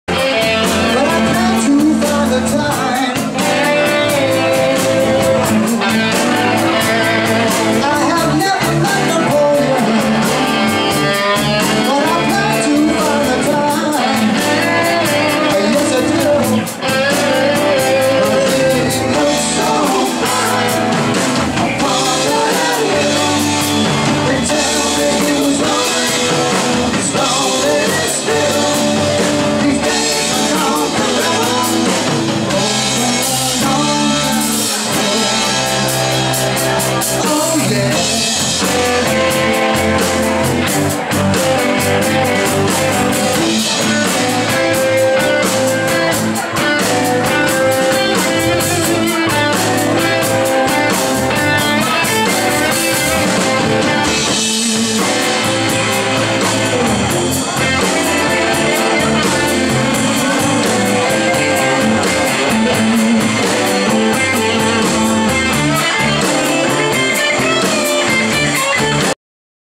Tangerine-Steely Dan-1
Tangerine at Bill Ash's on October 22, 2010